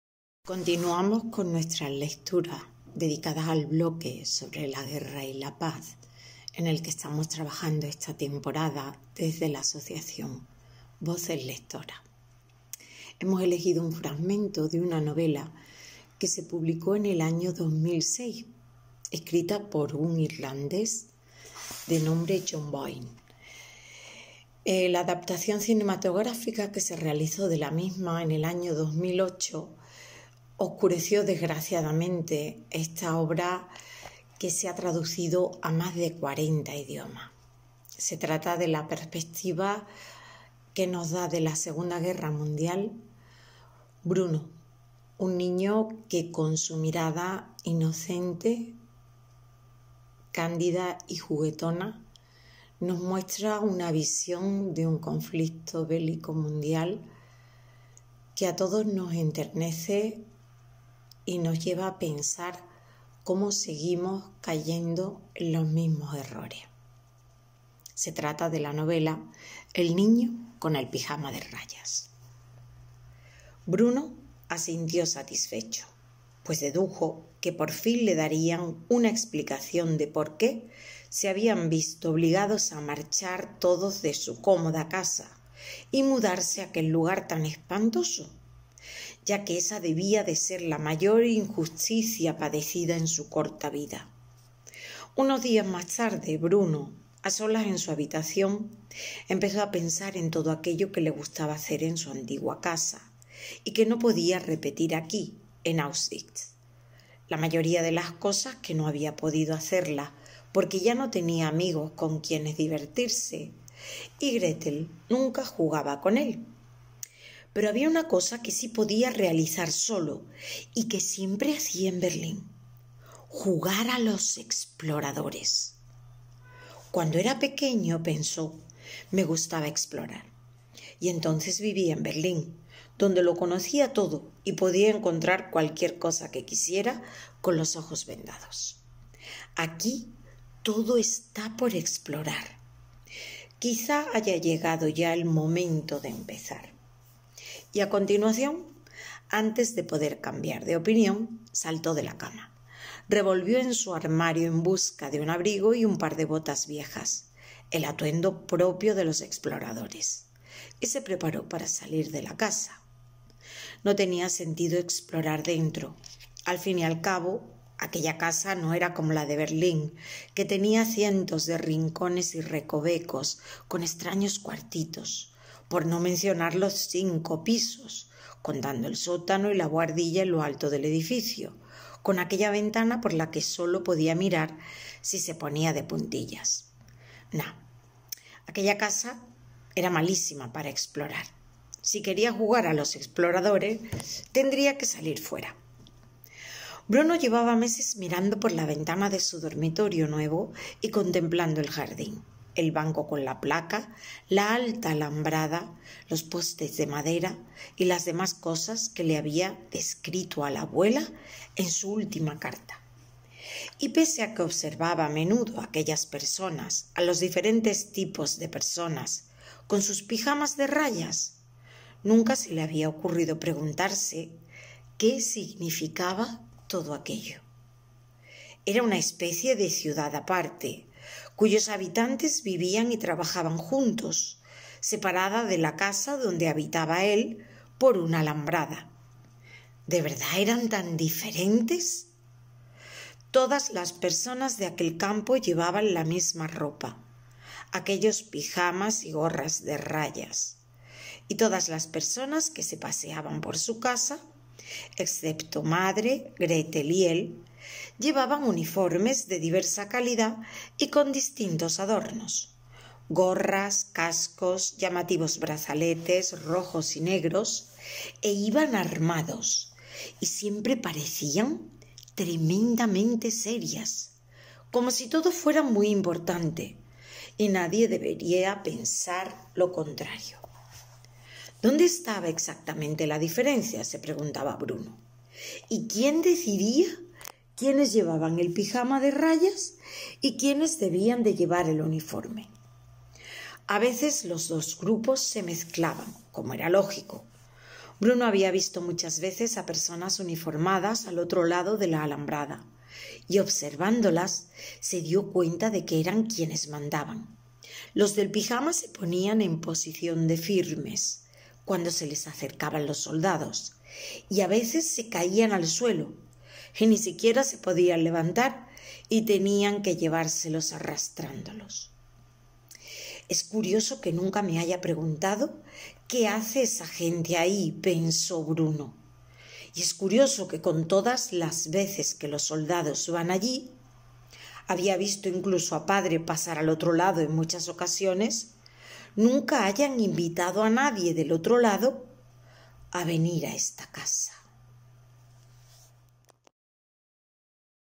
Audio Lectura 97. "El niño con el pijama de rayas" de John Boyne
“El niño con el pijama de rayas” es una de esas historias que hay que leer al menos una vez en la vida, sin prisas y con la certeza de que después de conocerla ya no volveremos a ver la vida de la misma manera. Publicada a inicios de 2006 por el autor irlandés John Boyne, narra la historia de la Alemania nazi y los campos de concentración a través del prisma de la amistad que surge entre dos niños.